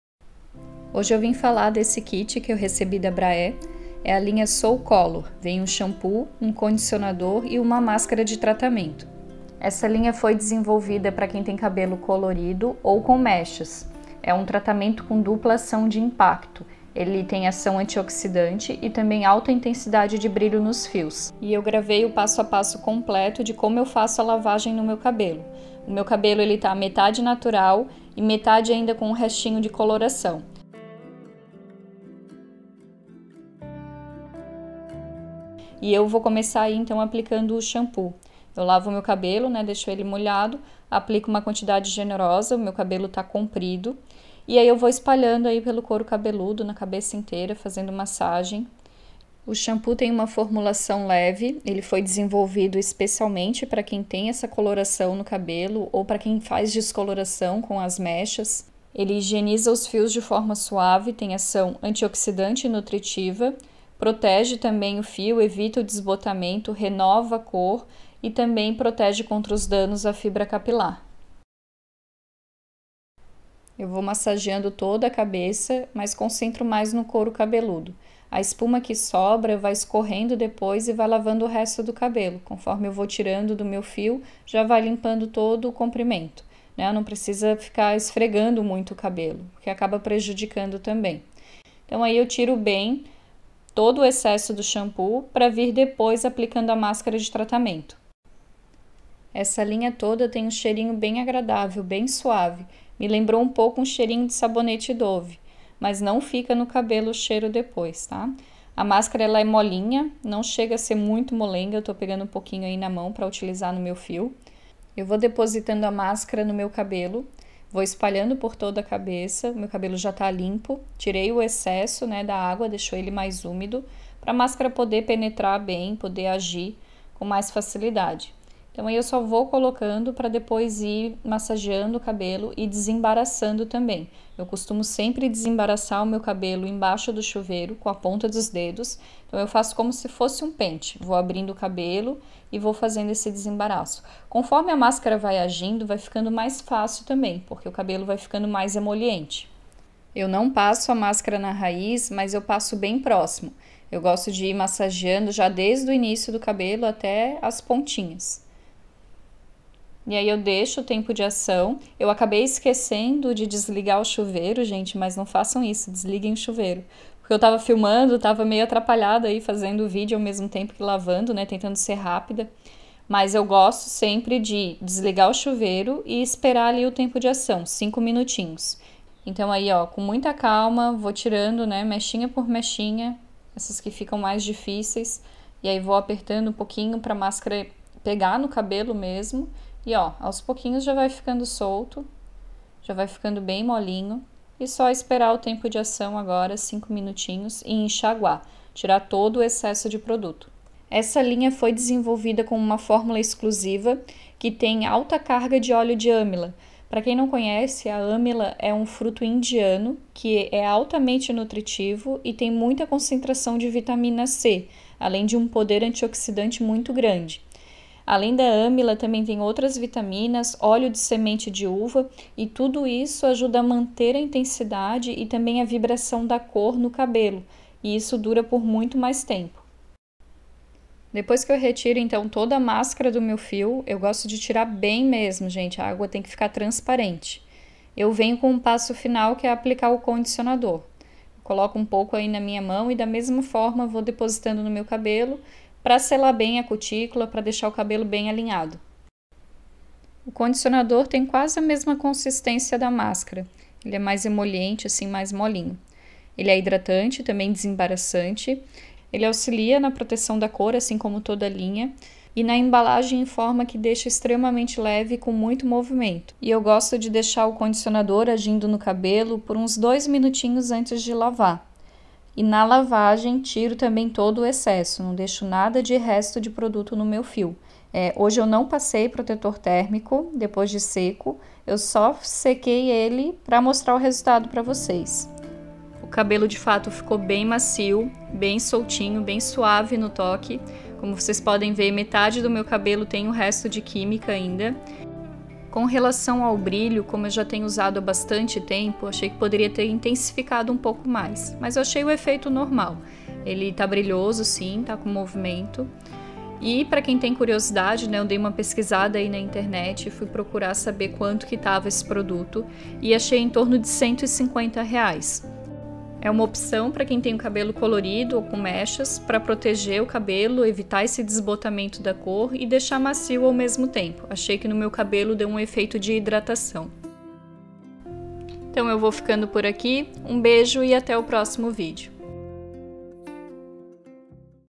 LINHA SOUL COLOR BRAÉ- Resenha
Testei a linha Soul Color da Braé para cabelos coloridos ou com mechas, tratamento para proteção da cor e renovação do brilho, através do óleo de Amla, vitaminas e óleo da semente de uva. Mostrei todo o passo a passo de como eu lavo o meu cabelo.